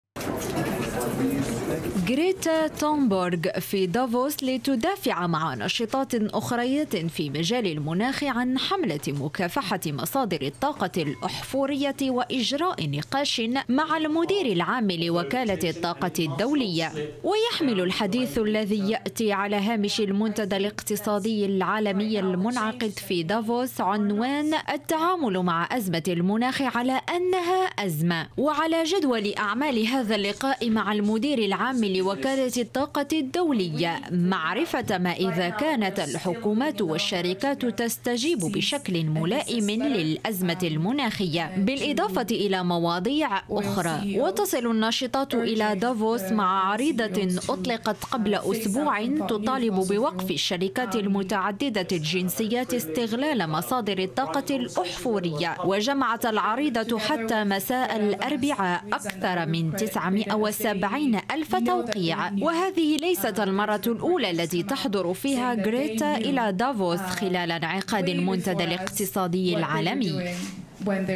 سويسرا.. غريتا تونبرغ في دافوس لمواصلة حملتها ضد مصادر الطاقة الأحفورية
لمتابعتنا

لتحميل التطبيق